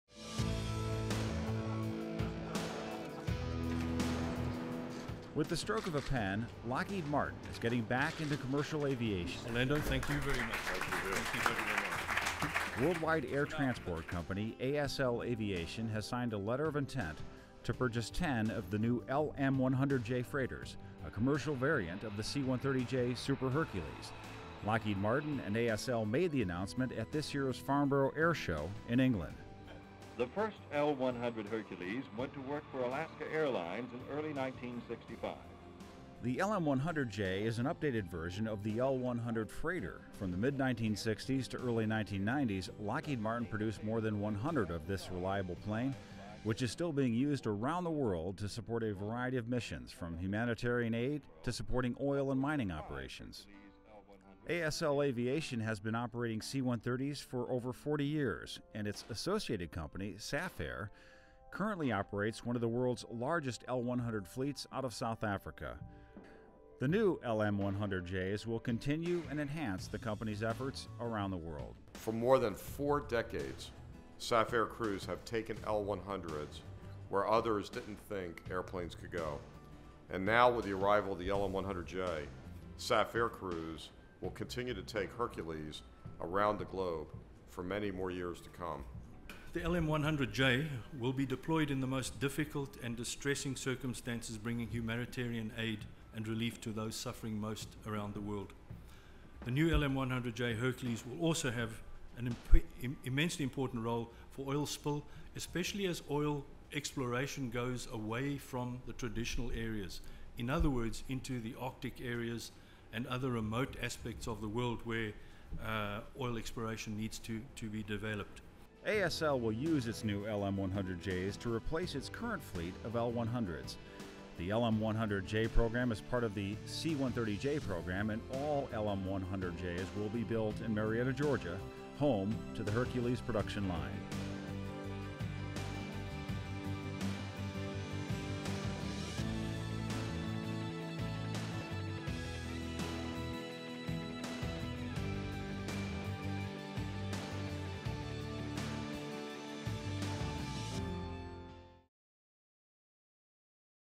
ASL Aviation Group Signs Letter of Intent To Procure Lockheed Martin LM-100J Freighters
ASL Aviation Group signed a Letter of Intent with Lockheed Martin to order up to 10 LM-100J commercial freighters during a ceremony on July 16, 2014, at the Farnborough International Airshow. The LM-100J is the civil-certified version of Lockheed Martin's proven C-130J Super Hercules and is an updated version of the L-100 (or L-382) cargo aircraft.